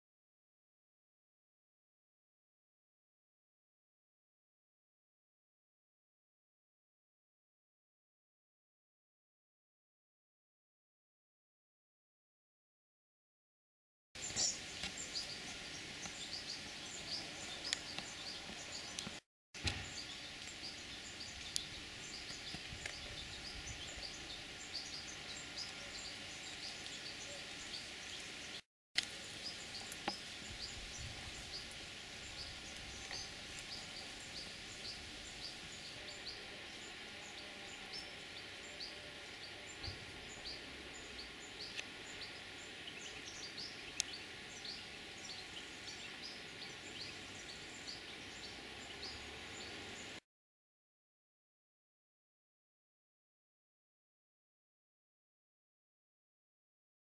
Lesser Purple Emperor Butterfly Apertura ilia male showing off his purple sheen, La Romieu, France
A male Lesser Purple Emperor Butterfly Apertura ilia of the typical form is displaying a lovely purple sheen as his wings catch the light on a rather breezy day in the south of France.